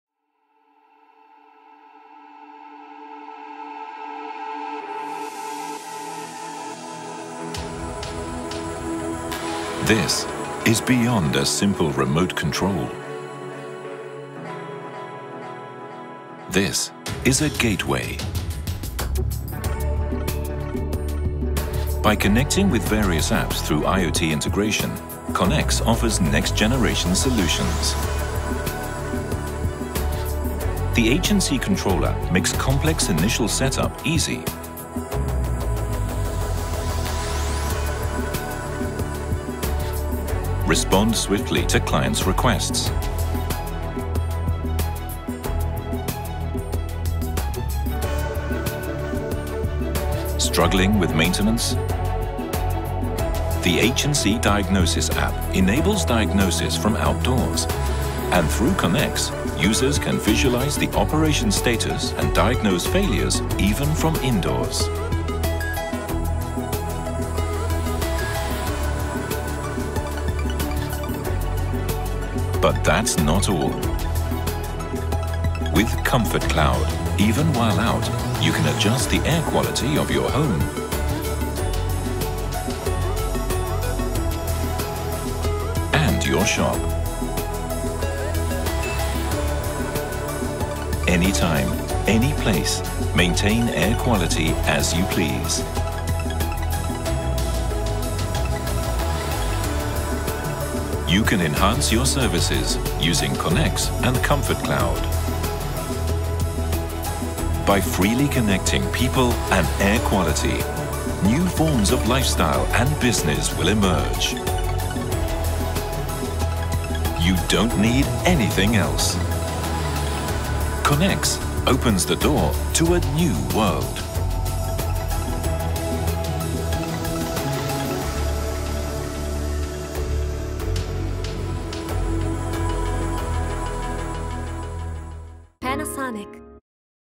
The NEW Generation CONEX
CONEX new IoT remote controller offers next generation solutions for PAC and VRF line-up.